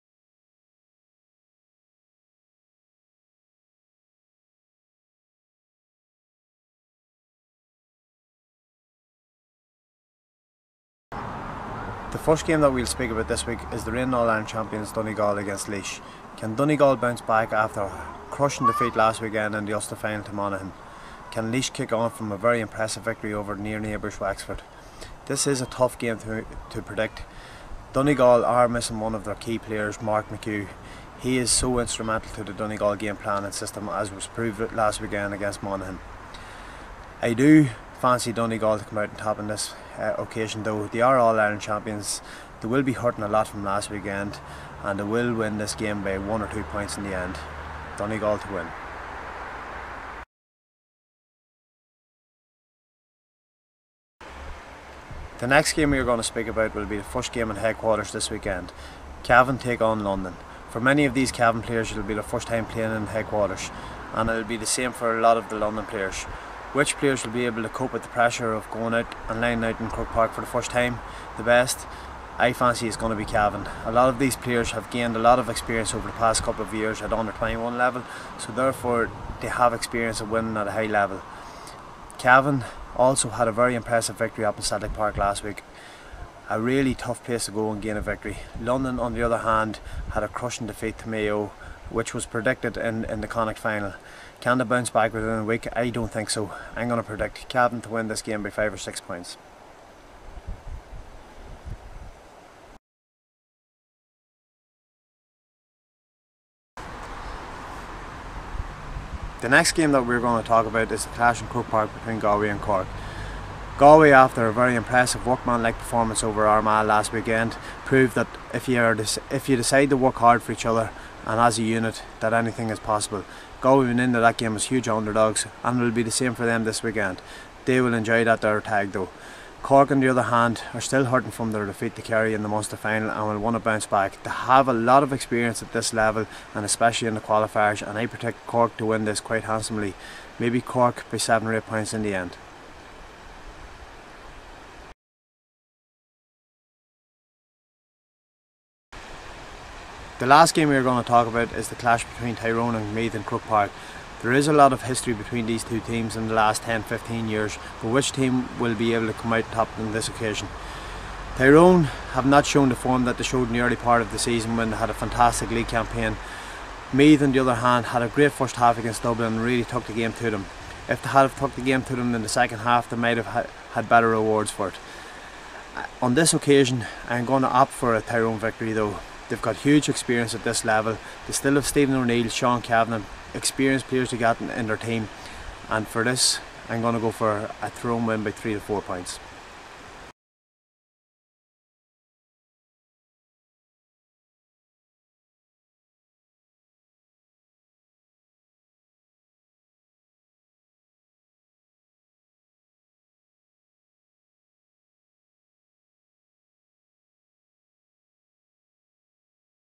Steven McDonnell GAA Tips Round 3
We are into our third week now of our staff member Steven McDonnells GAA predictions.

Week one he played a blinder and called 5 out of 5 right. Week 2 didn't go so well and he saw his predictions blown out of the water with even his beloved Armagh letting him down against Galway.

This week he is looking to recover some dignity and call a clean 4 out 4 right. This weekend we see Donegal V Laois, Cavan V London, Galway V Cork and Tyrone V Meath.

Check out the video and let us know if you agree with Stevie or if you believe he's another non-runner.